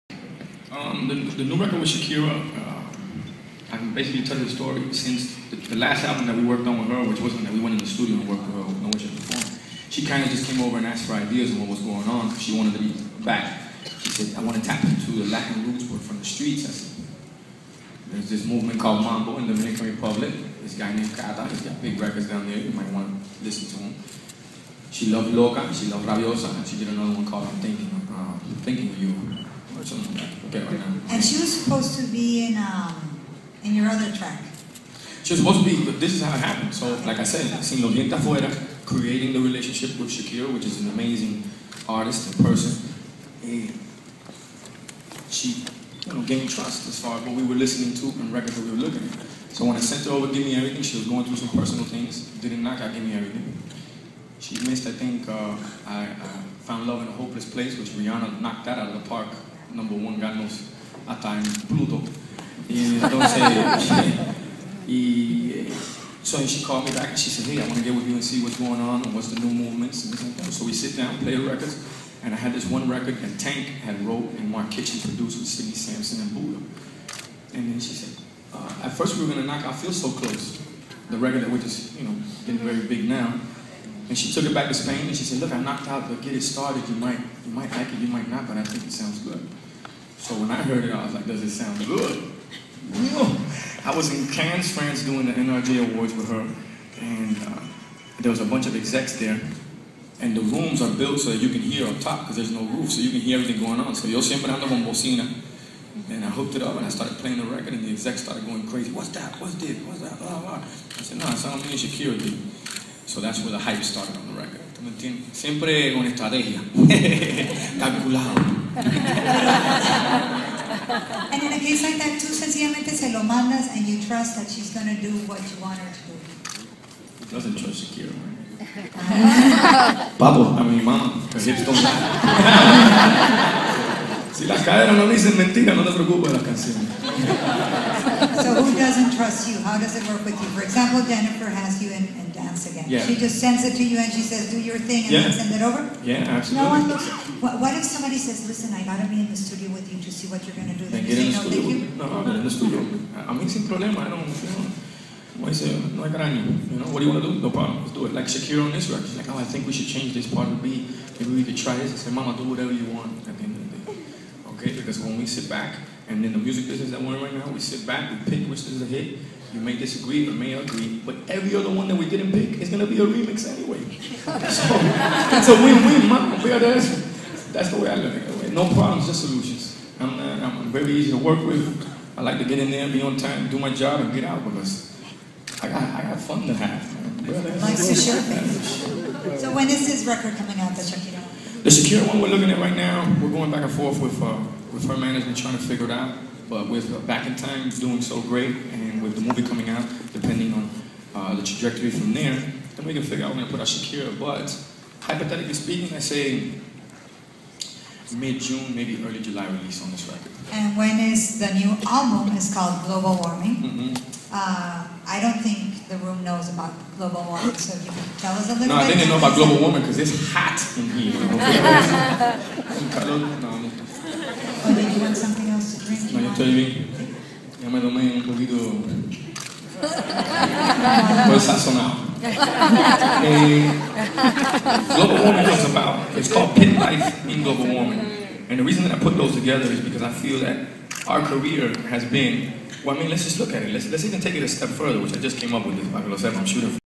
Pitbull, Prince Royce, Floyd The Rock Artist, Edesio Alejandro y Cristian Alejandro “Billboart 2012”
Pitbull, Prince Royce, Floyd The Rock Artist, Edesio Alejandro, Cristian Alejandro, Elizabeth Ortiz y Fleriser Bello at Billboard - Miami
Pitbull at the Billboard Latin Conference 2012.
Video by My LifeStyle Magazine - NY